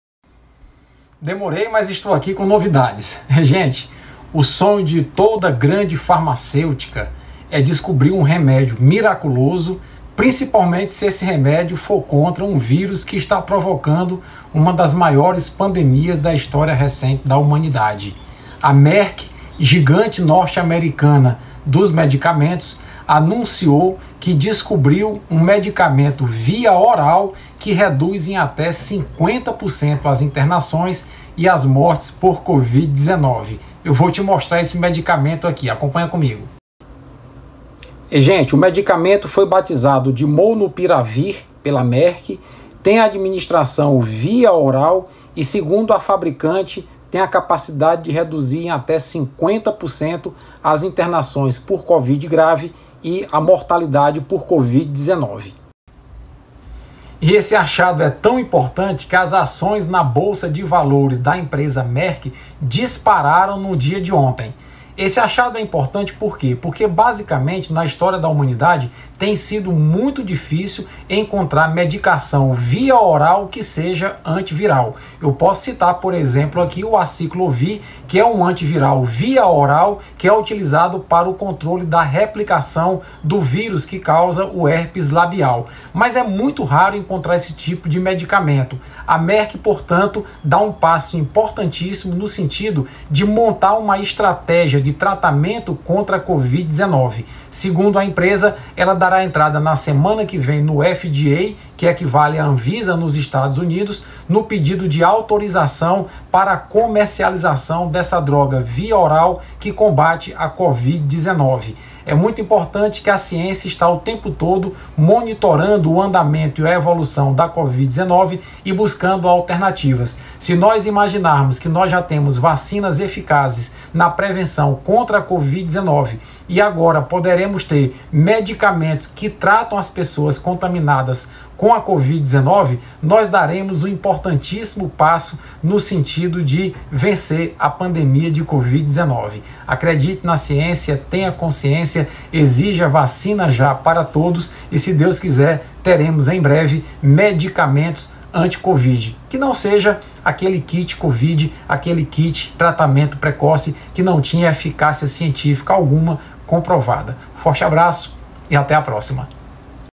MOLNUPIRAVIR: MERCK ENCONTRA A CURA DA COVID-19?
MOLNUPIRAVIR: MERCK encontra a cura da Covid-19? A farmacêutica MERCK anunciou resultados de uma pesquisa com mais de 700 voluntários com Covid-19 usando a droga molnupiravir. Os resultados apontaram eficácia de 50% na redução de internação e morte por Covid. Embalada pelos resultados a farmacêutica anunciou que irá pedir ao FDA autorização para uso em seres humanos. A vantagem do antiviral ser administrado via oral é bastante relevante pois dispensa internação e risco de infecção hospitalar.